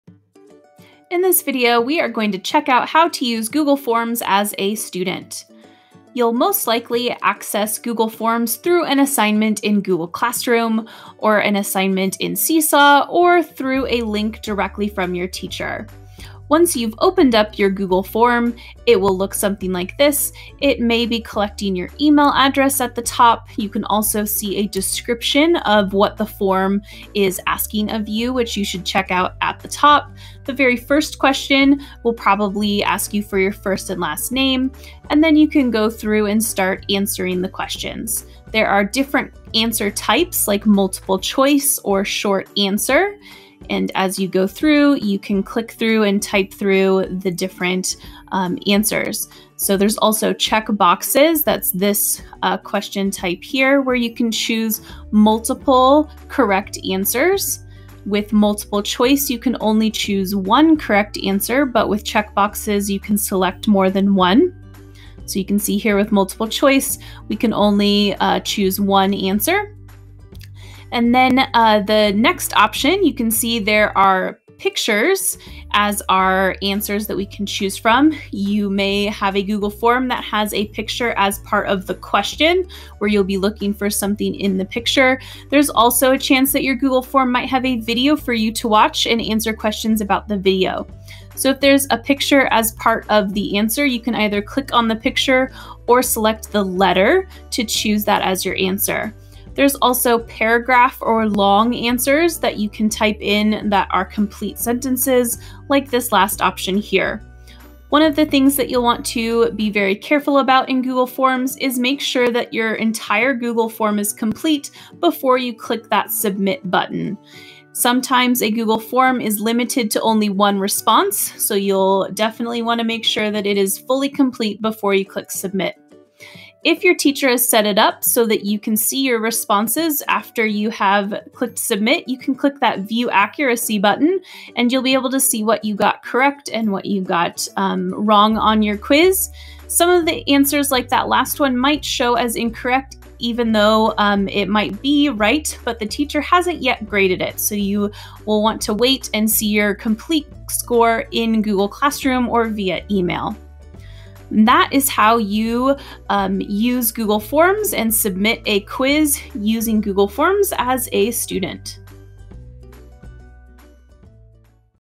How to Use Google Forms as a Student
In this video we walk through how to use Google Forms as a student.